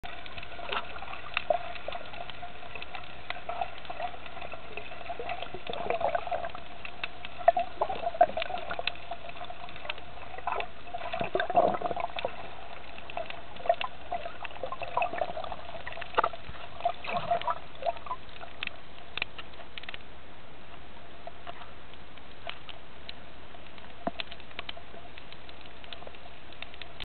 Spettacolare fondale vulcanico di Ponza
Video realizzato durante un'escursione a Ponza con i colleghi della Sapienza: durante una nuotata in snorkeling, è stato possibile osservare una ricca flora ed una altrettanto ricca flora; in questo video mi soffermo sulla spettacolarità delle rocce di origine vulcanica che costituiscono il fondale di Ponza.
Il video è stato girato a Ponza (Latina).